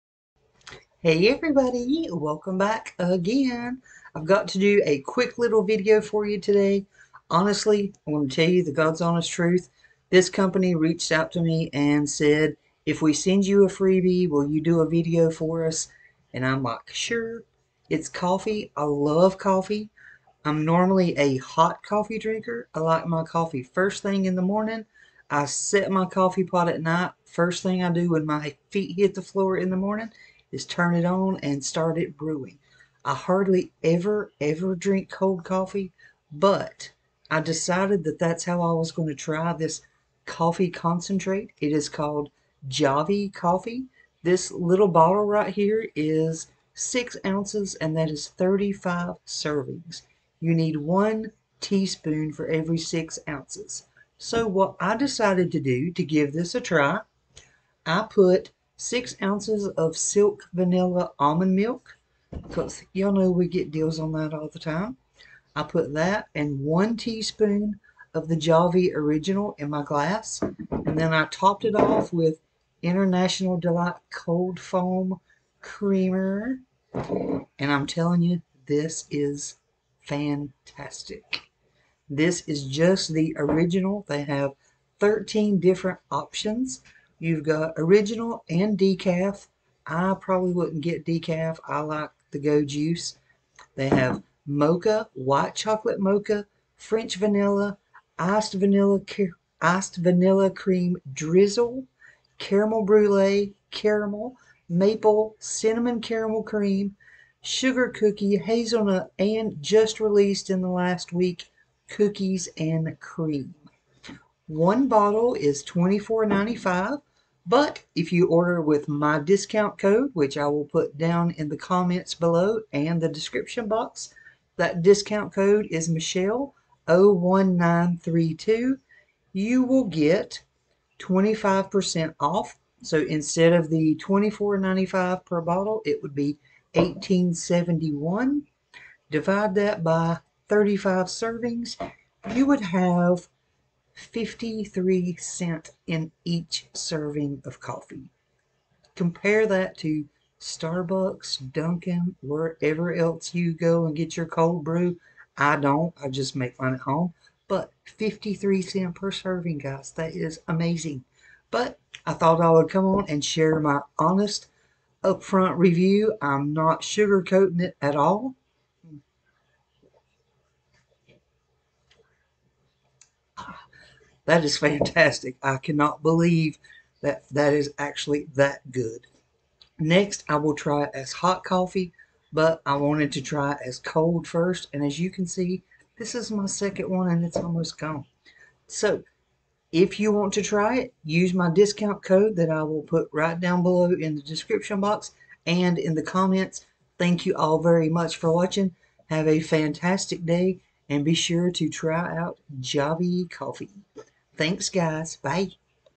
Be your own barista ☕️ Javy Coffee! Discount Code Included!! 2/26 @javvycoffee
Javy Coffee! It's amazing! Honest review! Discount Code Included!! 2/26
It's absolutely delicious!

Gluten-free, Dairy-free, Sustainably sourced, Ethically sourced

Discount Code for 25% off: Michelle01932